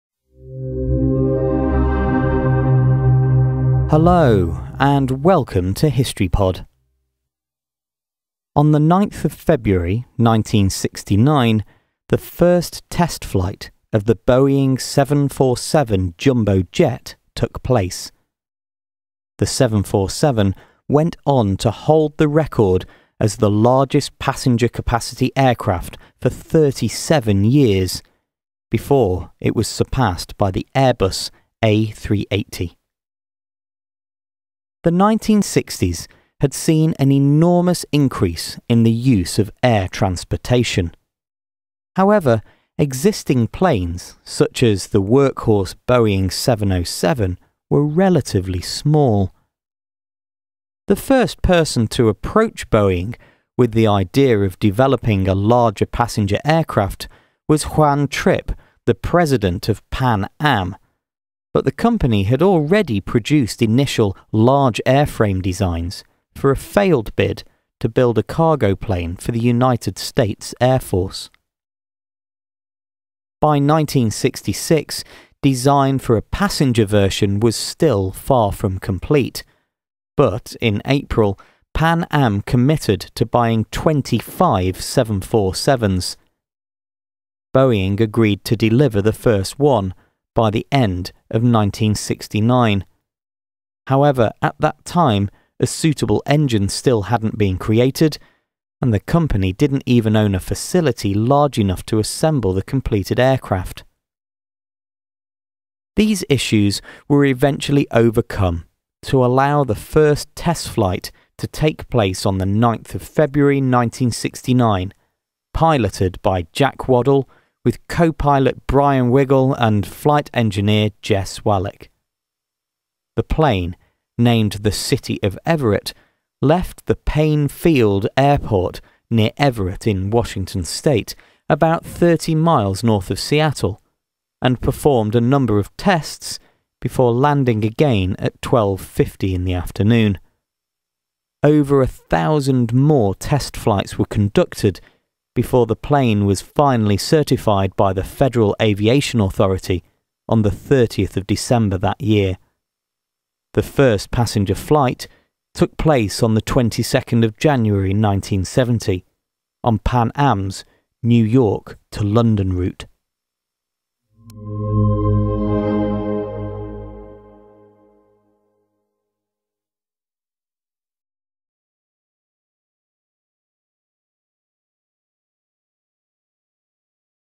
9th February 1969: First test flight of Boeing 747 'Jumbo Jet'
The 747 went on to hold the record for the largest passenger capacity for 37 years before being surpassed by the Airbus A380.

The 1960s saw an enormous increase in the use of air transportation. However, existing planes such as the Boeing 707 were relatively small. The first person to approach Boeing with the idea of developing a larger passenger aircraft was Juan Trippe, president of Pan Am, but the company had already produced initial large airframe designs for a failed bid to build a cargo plane for the United States Air Force.

By 1966 the design was still far from complete but, in April, Pan Am committed to buying twenty-five 747s. Boeing agreed to deliver the first one by the end of 1969. However a suitable engine still hadn’t been created, and the company didn’t even own a facility large enough to assemble the completed aircraft.

These issues were eventually overcome to allow the first test flight to take place on the 9th February 1969, piloted by Jack Waddell with co-pilot Brien Wygle and flight engineer Jess Wallick. The plane, named the City of Everett, left the Paine Field near Everett in Washington State – about 30 miles north of Seattle – and performed a number of tests before landing again at 12.50pm. Over 1,000 more test flights were conducted before the plane was certified by the Federal Aviation Authority on the 30th December. The first passenger flight took place on the 22nd January 1970, on Pan Am's New York to London route.